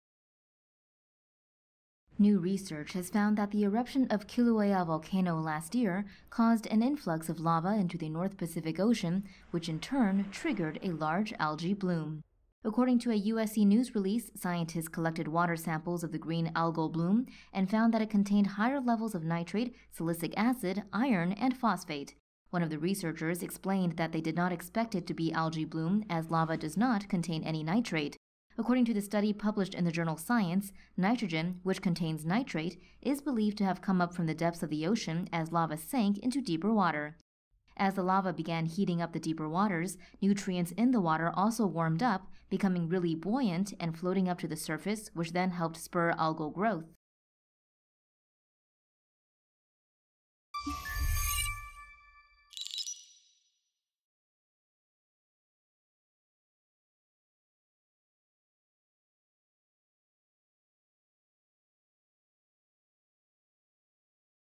Massive algae bloom spurred by lava from Kilauea eruption
New research has found that the Kilauea eruption last year caused an influx of lava into the North Pacific Ocean which in turn triggered a large algae bloom.

Satellite images from NASA showed the water surrounding the Kilauea volcano starting to turn green soon after the volcanic eruption.

Scientists collected water samples of the green algal bloom and found that it contained higher levels of nitrate, silicic acid, iron and phosphate, according to a news release from the University of Southern California.

One of the researchers involved with the study explained that they did not expect an algae bloom as lava does not contain any nitrate.

Nitrogen, which contains nitrate, is believed to have come up from the depths of the ocean as lava sank into deeper water, according to the study published in the journal Science.

As the lava began heating up the deeper waters, the nutrients in the water also warmed up, becoming really buoyant and floating up to the surface, which then helped spur algal growth.

According to Hawaii Tribune-Herald, green algae is important as fish and other marine creatures in the sea are dependent on it for nutrients.

The authors of the study suggested that it is possible this mechanism has led to similar ocean fertilization events in the past associated with the formation of the Hawaiian Islands and other major volcanic eruptions.

RUNDOWN SHOWS:
1. Kilauea volcano exploding and lava entering the ocean
2. What water samples from the ocean contained
3. Cross section of ocean
4. How algae growth was spurred

VOICEOVER (in English):
"New research has found that the eruption of Kilauea volcano last year caused an influx of lava into the North Pacific Ocean which in turn triggered a large algae bloom."

"According to a USC news release, scientists collected water samples of the green algal bloom and found that it contained higher levels of nitrate, silicic acid, iron and phosphate."

"One of the researchers explained that they did not expect it to be algae bloom as lava does not contain any nitrate."

"According to the study published in the journal Science, nitrogen, which contains nitrate, is believed to have come up from the depths of the ocean as lava sank into deeper water."

SOURCES: University of Southern California, Science, Hawaii Tribune-Herald, West Hawaii Today